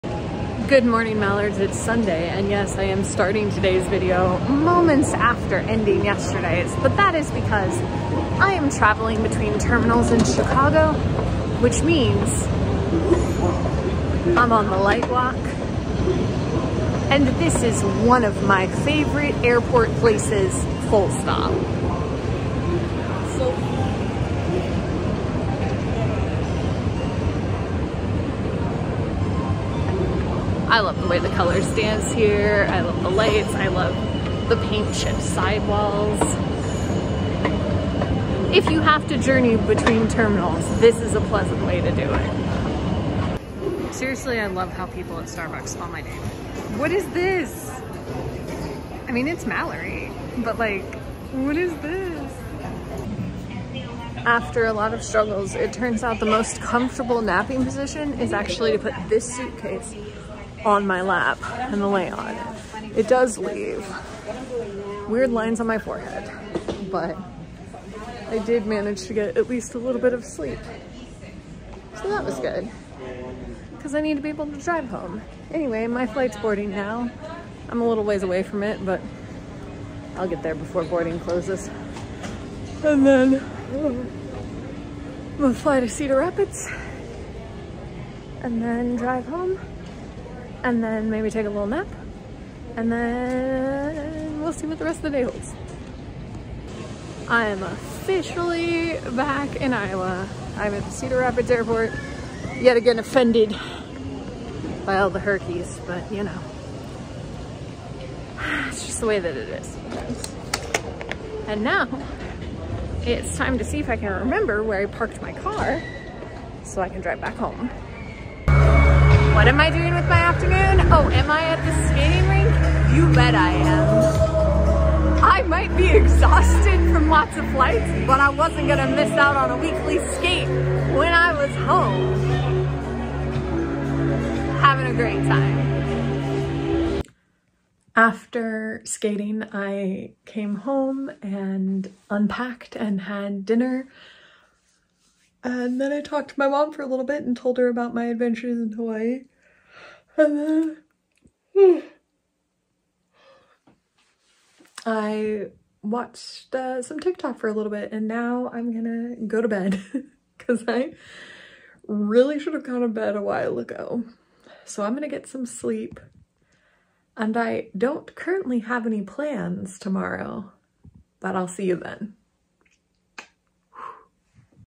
The Exhausted End Of A Trip
In which I finally made it home and promptly went skating.